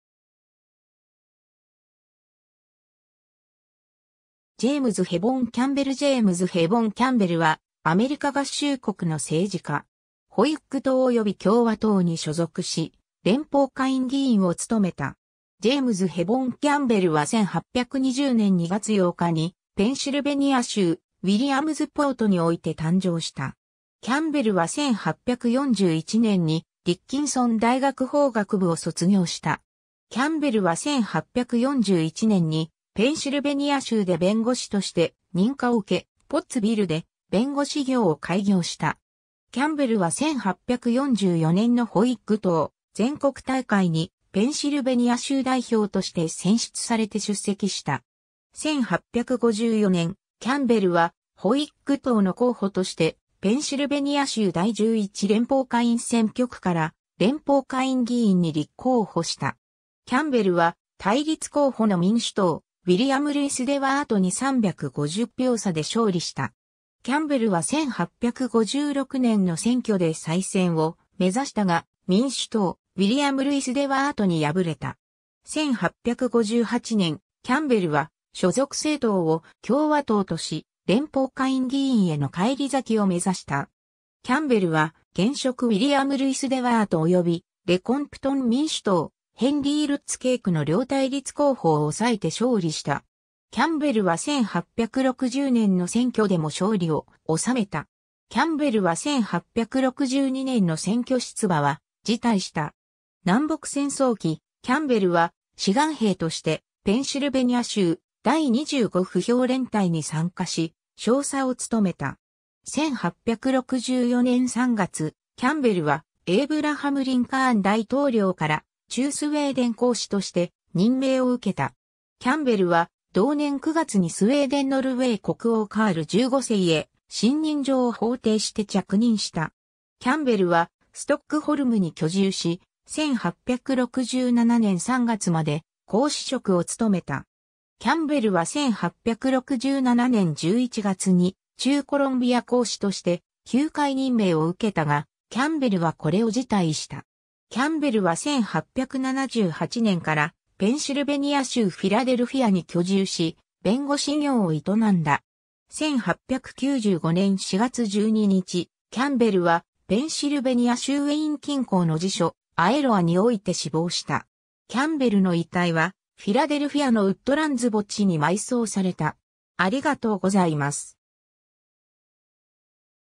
ジェイムズ・ヘボン・キャンベル
ジェイムズ・ヘボン・キャンベル ジェイムズ・ヘボン・キャンベル（James Hepburn Campbell, 1820年2月8日 - 1895年4月12日）は、アメリカ合衆国の政治家。
ホイッグ党および共和党に所属し、連邦下院議員を務めた。
 ジェイムズ・ヘボン・キャンベルは1820年2月8日にペンシルベニア州ウィリアムズポートにおいて誕生した。
キャンベルは1841年にディッキンソン大学法学部を卒業した。
キャンベルは1841年にペンシルベニア州で弁護士として認可を受け、ポッツビルで弁護士業を開業した。
キャンベルは1844年のホイッグ党全国大会にペンシルベニア州代表として選出されて出席した。
 1854年、キャンベルはホイッグ党の候補として、ペンシルベニア州第11連邦下院選挙区から連邦下院議員に立候補した。
キャンベルは対立候補の民主党ウィリアム・ルイス・デワートに350票差で勝利した。
キャンベルは1856年の選挙で再選を目指したが、民主党ウィリアム・ルイス・デワートに敗れた。
 1858年、キャンベルは所属政党を共和党とし、連邦下院議員への返り咲きを目指した。
キャンベルは現職ウィリアム・ルイス・デワートおよびレコンプトン民主党ヘンリー・ルッツ・ケイクの両対立候補を抑えて勝利した。
キャンベルは1860年の選挙でも勝利を収めた。
キャンベルは1862年の選挙出馬は辞退した。
 南北戦争期、キャンベルは志願兵としてペンシルベニア州第25歩兵連隊に参加し、少佐を務めた。
 1864年3月、キャンベルはエイブラハム・リンカーン大統領から駐スウェーデン公使として任命を受けた。
キャンベルは同年9月にスウェーデン＝ノルウェー国王カール15世へ信任状を奉呈して着任した。
キャンベルはストックホルムに居住し、1867年3月まで公使職を務めた。
キャンベルは1867年11月に駐コロンビア公使として休会任命を受けたが、キャンベルはこれを辞退した。
 キャンベルは1878年からペンシルベニア州フィラデルフィアに居住し、弁護士業を営んだ。
1895年4月12日、キャンベルはペンシルベニア州ウェイン近郊の地所「アエロア」において死亡した。
キャンベルの遺体はフィラデルフィアのウッドランズ墓地に埋葬された。